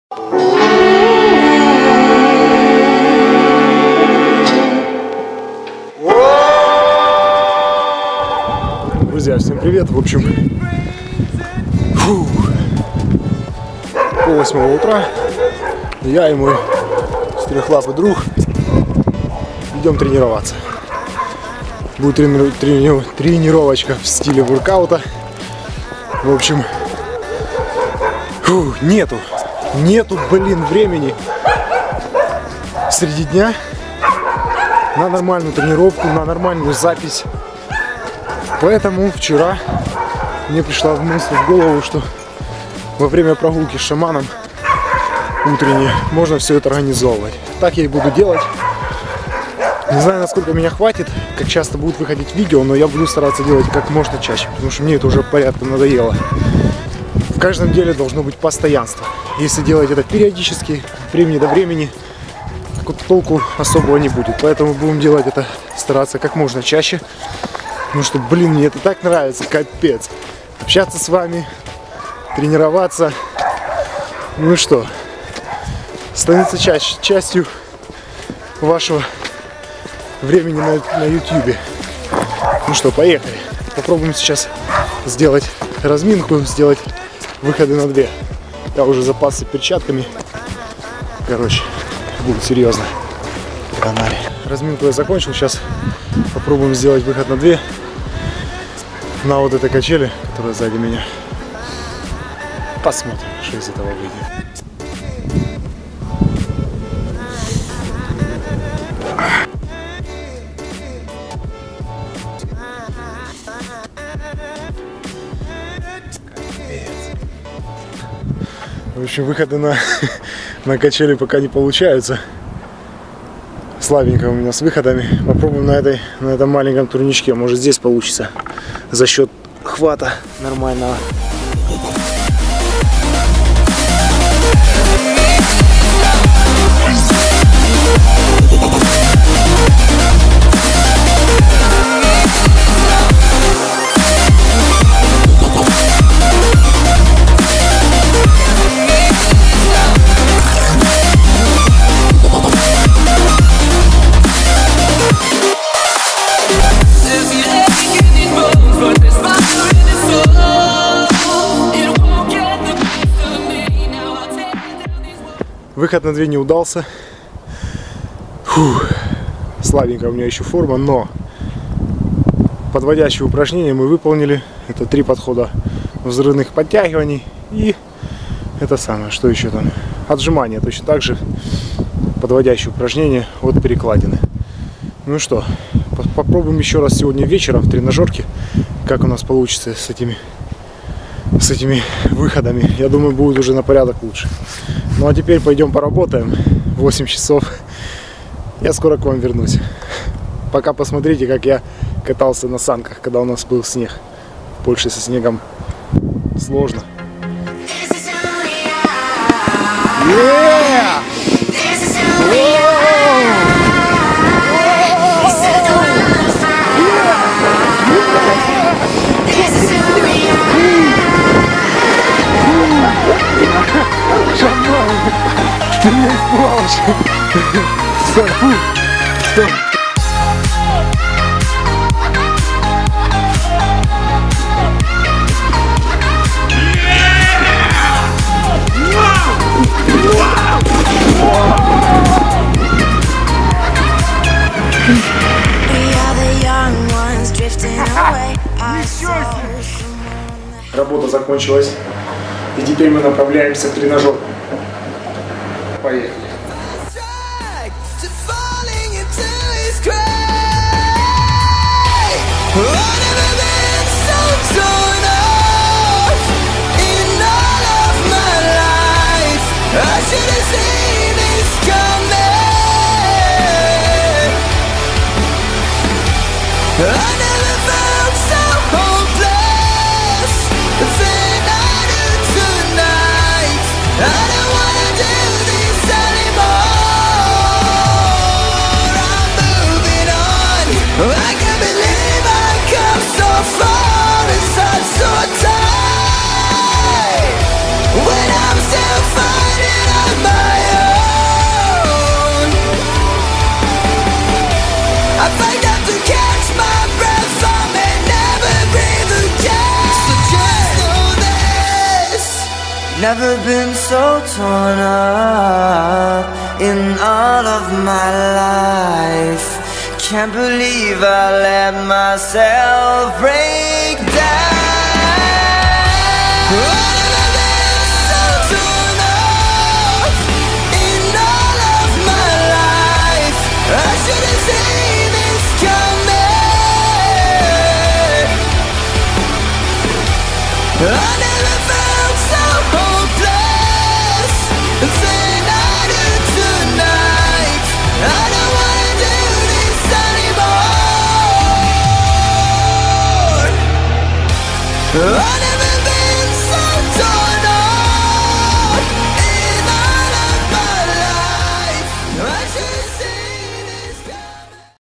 ВЫХОД НА ДВЕ!! Я МОГУ!
ВСЕМ ПРИВЕТ! Просто смотрите, как мне в итоге удалось сделать выход на две!